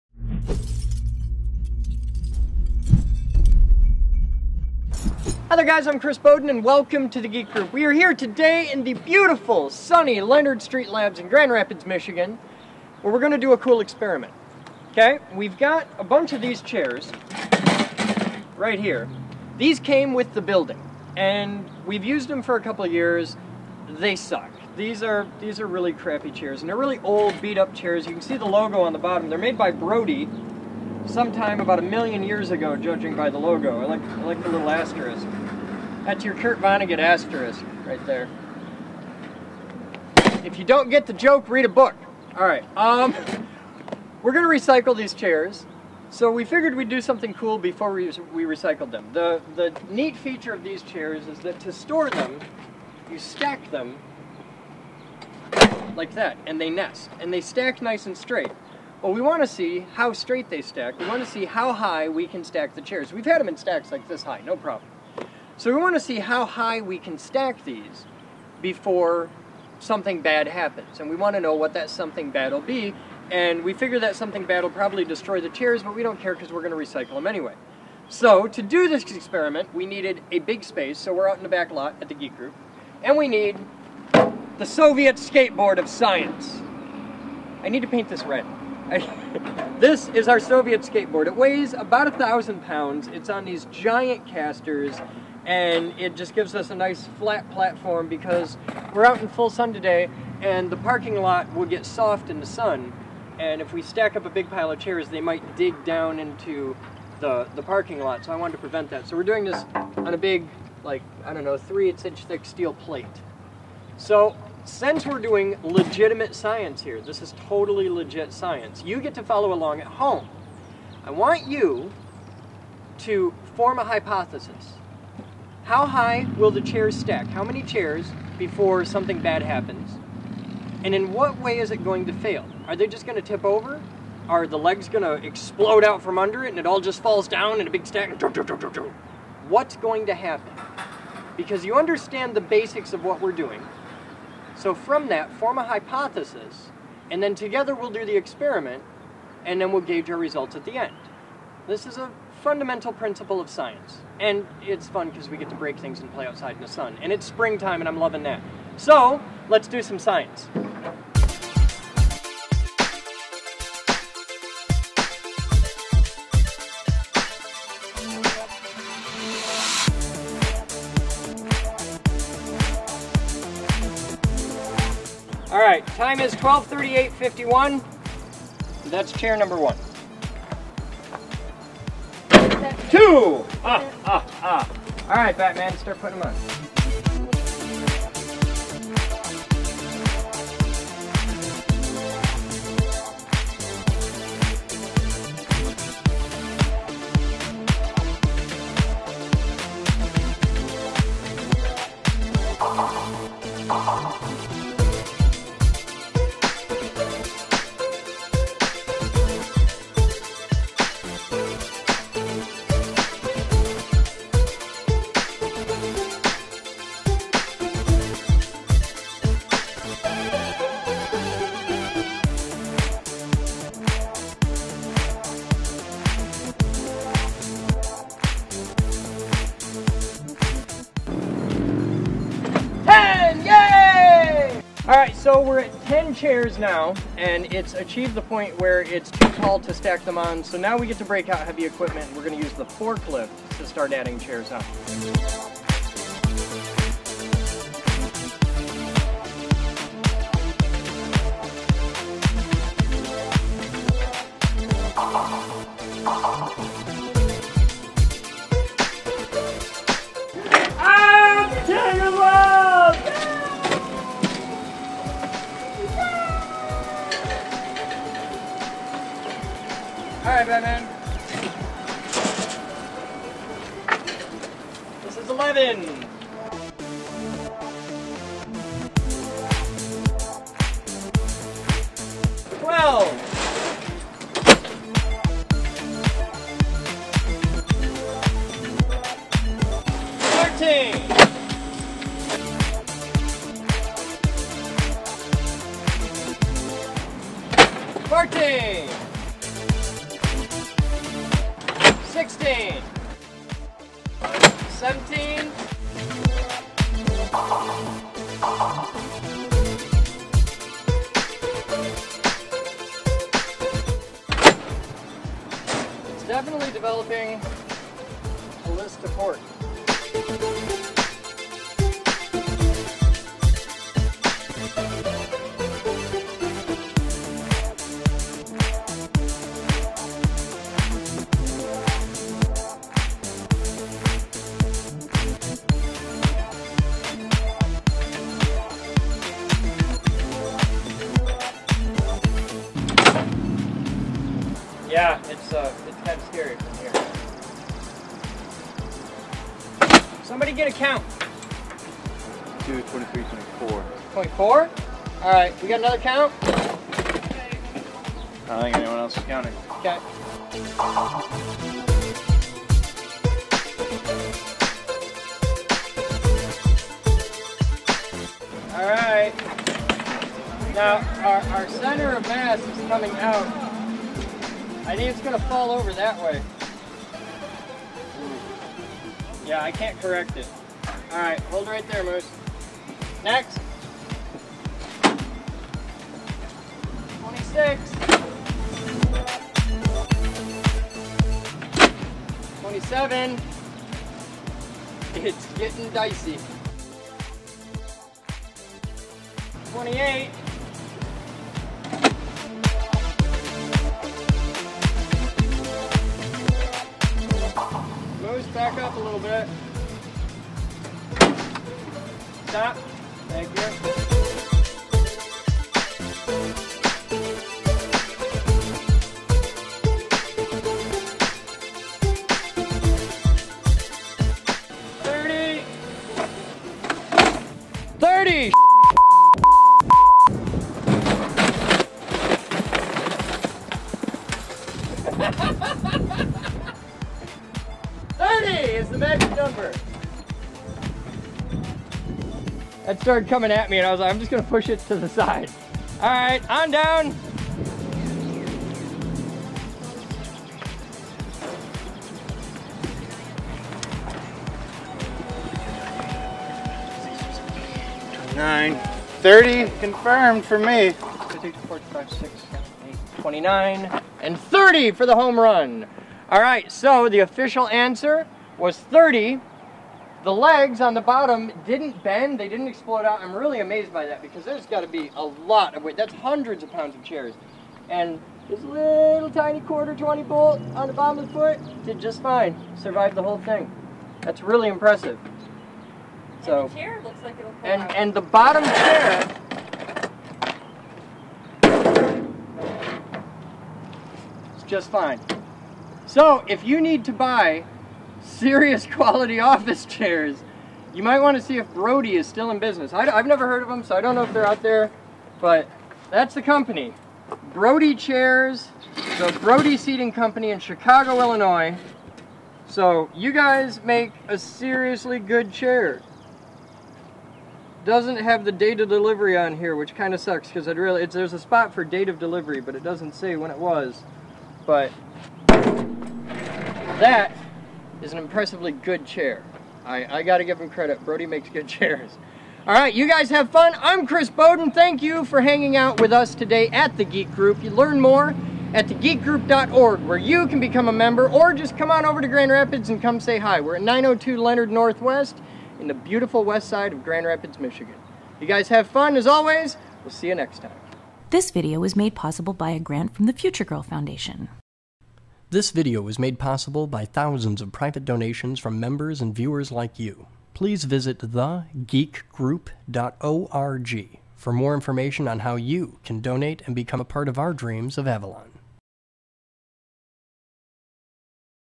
Chair Stacking Experiment
- As we were recycling a large amount of old chairs, Chris decided to carry out a little science experiment. These chairs stack together when stored. We often wondered just how high you could stack them. Since they were being removed from service, it was time to find out!  Starting with level surface, the chairs where stacked one by one. When they got above a safe height, we broke out the fork lift with the man lift attached. As we broke past about 20 chairs, it was clear what the point of failure might be. Watch the video and try to guess the amount of chairs and at what point they fail. Whilst a bit of fun, experiments like these help you think about physics and how it relates to just about everything. How could we have used physics to predict the probably outcome?